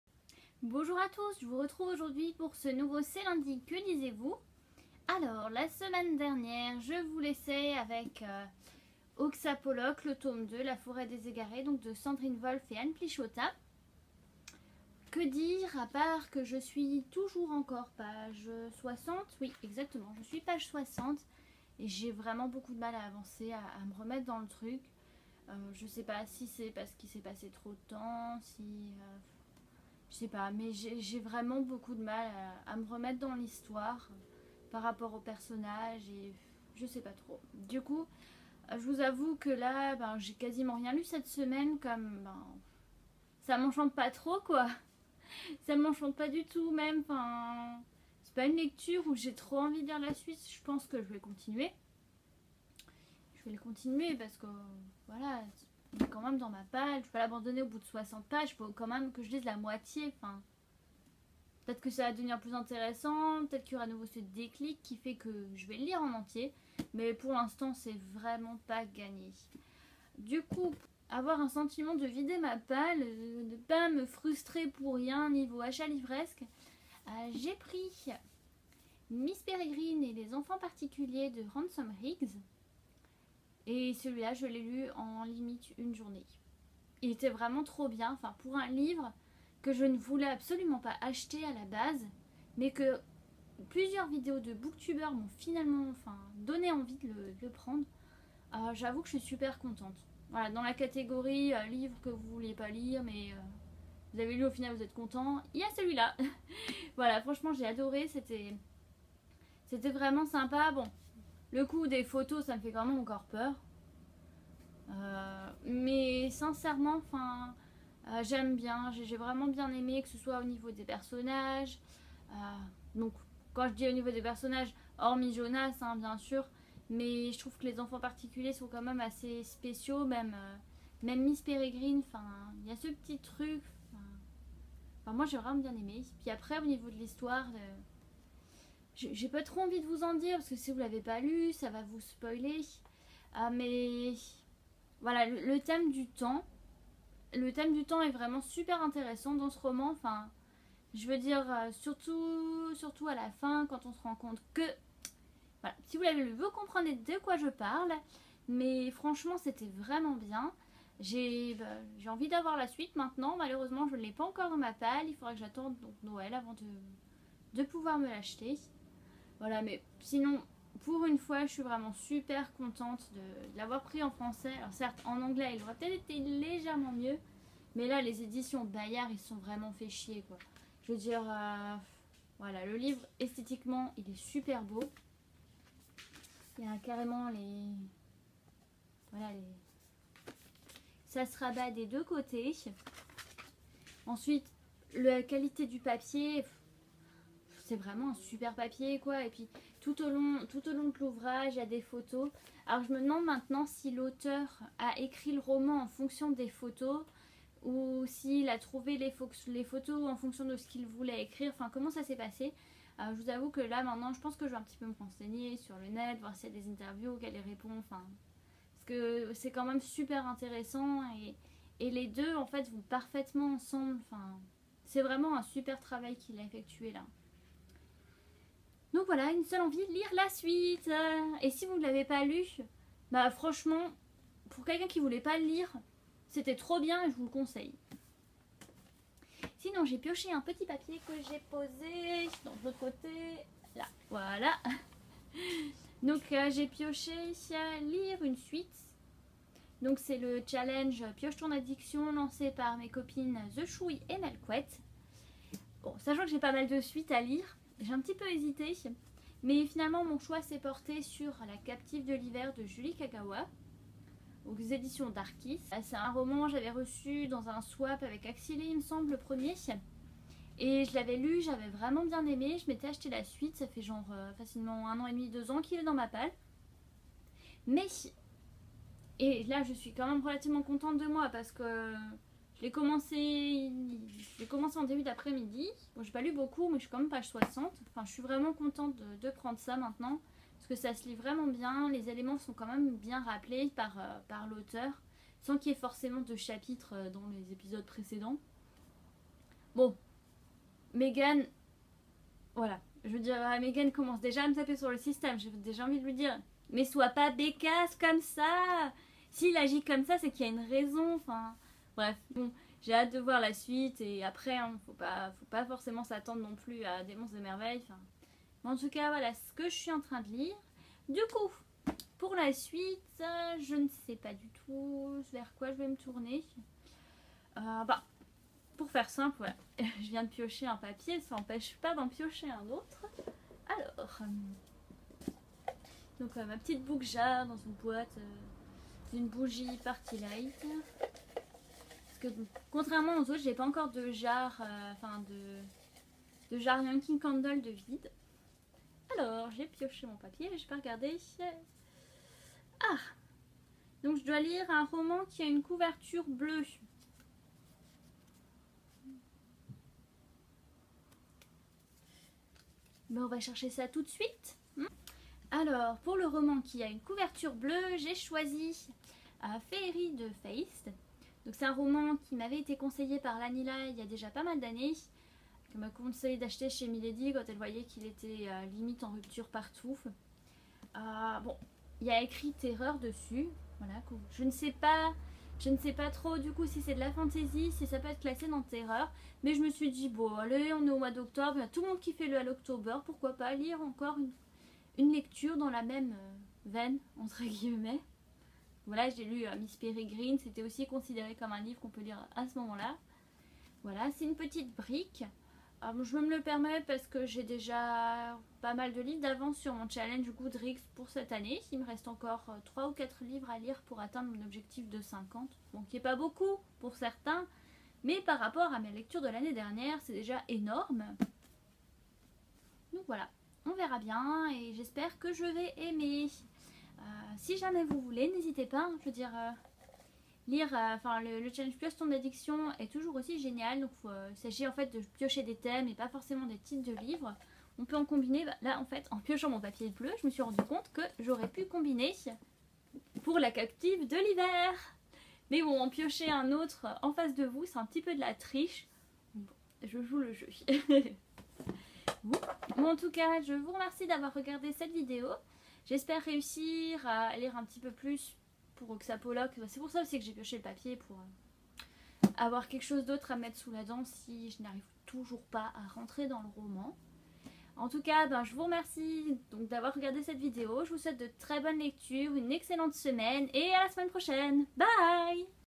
C'est lundi, que lisez-vous ? #11
Vidéo sur mes lectures passées, en cours et à venir.